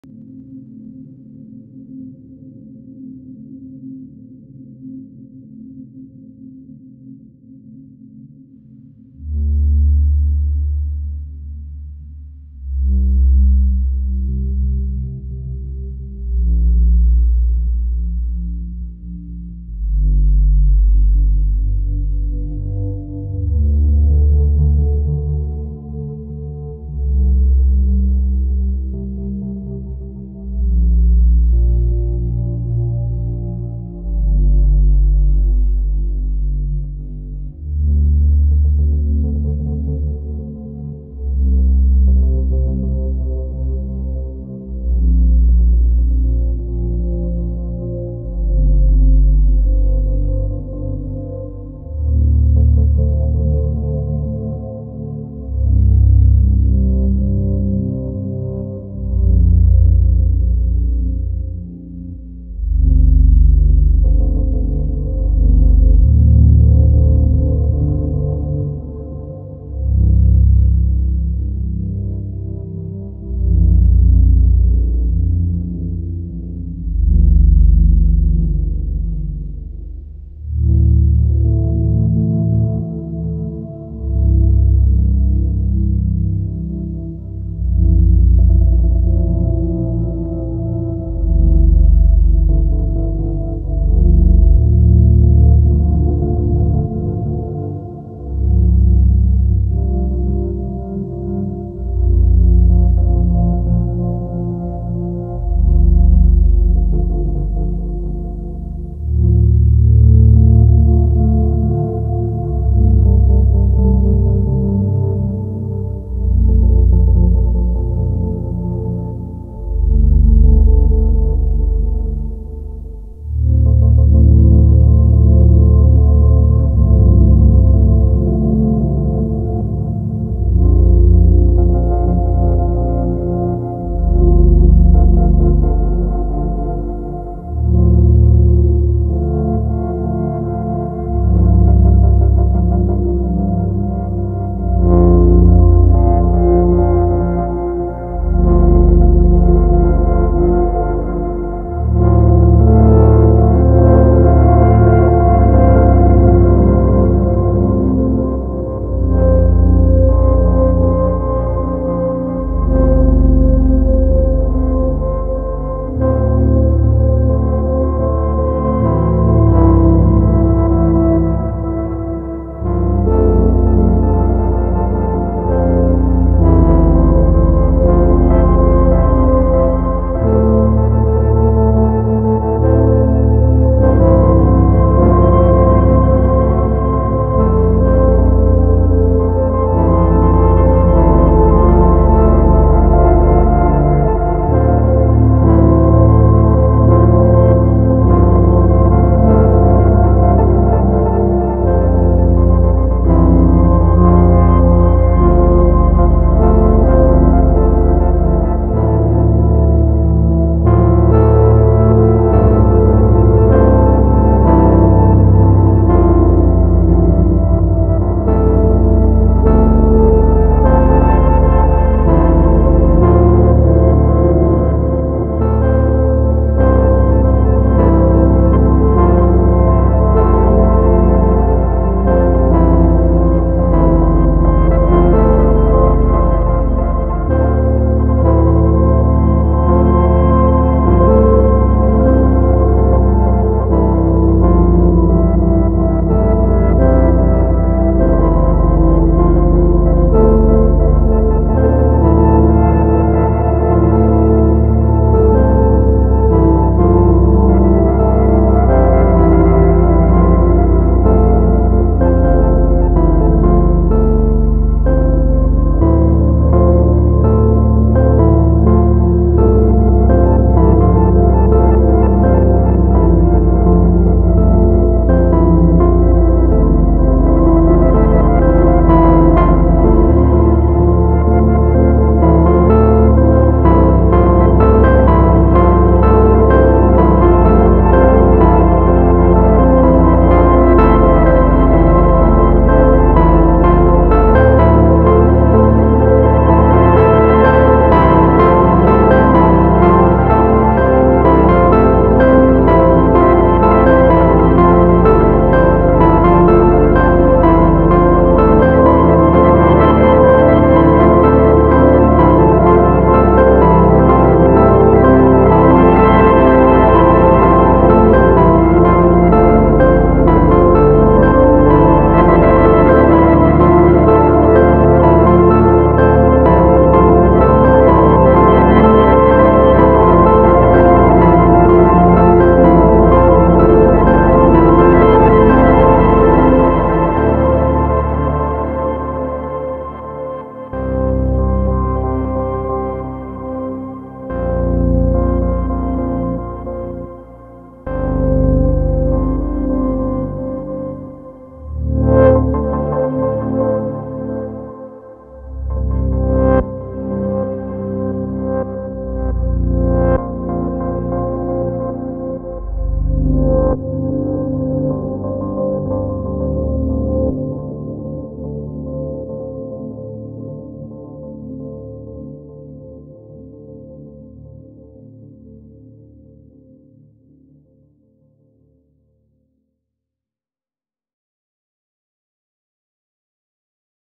Live Performance W/ Moog Subharmonicon & Hologram Microcosm
The Moog Subharmonicon is an inspiring semi-modular synthesizer with some tricks up it's sleeve.  Adding an effects pedal like the Hologram Microcosm takes it to another level.

Headphones or monitors recommended for the low end goodness.